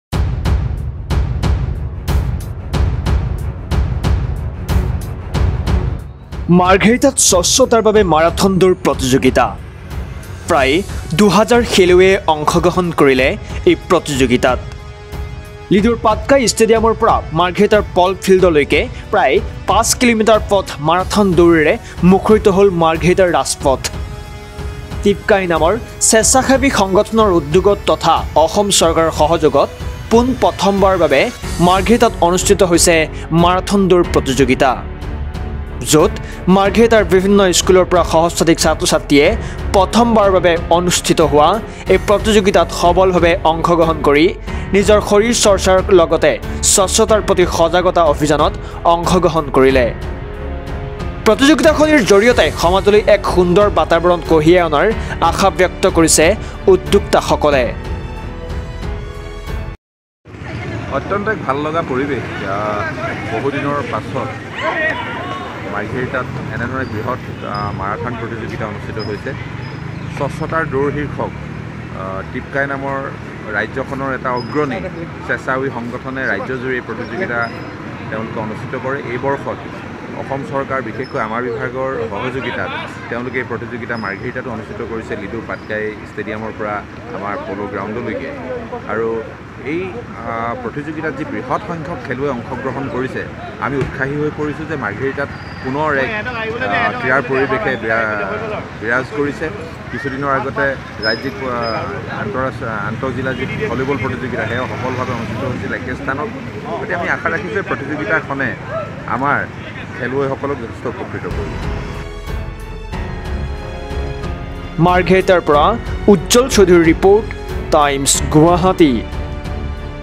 মাৰ্ঘেৰিটাত স্বচ্ছতাৰ বাবে মাৰাথন দৌৰ প্ৰতিযোগীতা : বহু খেলুৱৈয়ে অংশগ্ৰহন কৰিলে এই প্ৰতিযোগীতাত
মাৰ্ঘেৰিটাত স্বচ্ছতাৰ বাবে মাৰাথন দৌৰ প্ৰতিযোগীতা  : প্ৰায় দুহাজাৰ খেলুৱৈয়ে অংশগ্ৰহন কৰিলে এই প্ৰতিযোগীতাত ৷৷

আজি লিডুৰ পাটকাই ষ্টুডিয়ামৰ পৰা মাৰ্ঘেৰিটাৰ পল ফিল্ডৰ লৈকে প্ৰায় ৫ কিলোমিটাৰ পথ মাৰাথন দৌৰেৰে মুখৰিত হল মাৰ্ঘেৰিটাৰ ৰাজপথ ৷ টিপকাই নামৰ স্বেচ্ছাসেৱী সংগঠনৰ উদ্যোগত তথা অসম চৰকাৰৰ সহযোগত পুন প্ৰথম বাৰৰ বাবে মাৰ্ঘেৰিটাত অনুস্থিত হৈছে মাৰাথন দৌৰ প্ৰতিযোগীতা ৷৷ যত মাৰ্ঘেৰিটাৰ বিভিন্ন স্কুলৰ পৰা সহস্ৰাধিক ছাত্ৰ ছাত্ৰীয়ে  প্ৰথমবাৰৰ বাবে অনুস্থিত হোৱা এই প্ৰতিযোগীতাত সবল ভাৱে অংশগ্ৰহন কৰি  নিজৰ শৰীৰ চৰ্চাৰ লগতে  স্বচ্ছতাৰ প্ৰতি সজাগতা অভিযানত অংশগ্ৰহন কৰে ৷ প্ৰতিযোগীতা খনিৰ জড়িয়তে সমাজলৈ এক সুন্দৰ বাতাবৰন কঢ়িয়াই অনাৰ আশা ব্যক্ত কৰিছে উদ্যোক্তা সকলে ৷৷

উজ্জ্বল চৌধুৰী,  ( মাৰ্ঘেৰিটা)
timesguwahati news margherita ✅Follow Pulak Nath as Your friend at Facebook